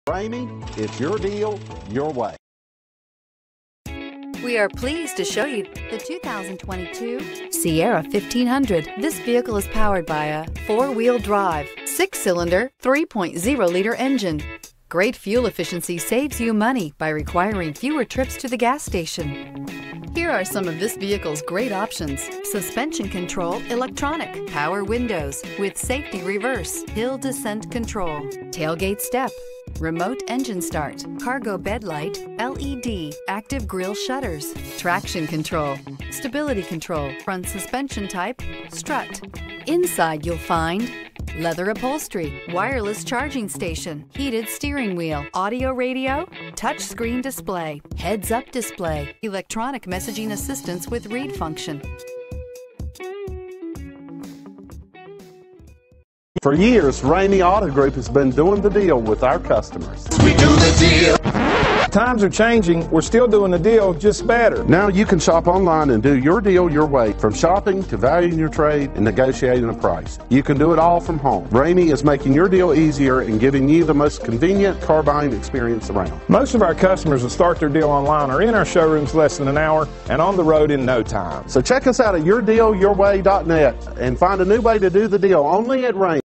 2022 GMC Sierra 1500 Princeton WV 1-221442
2022 GMC Sierra 1500 Denali Ultimate

Ramey Motors
127 Frazier Dr

2022 GMC Sierra 1500 Denali Onyx Black 4WD 10-Speed Automatic 3.0L I6 Lifetime no charge WV or VA Inspection Stickers at Ramey Auto Group!, 10-Speed Automatic, 4WD, Alpine Umber Leather.  22/26 City/Highway MPG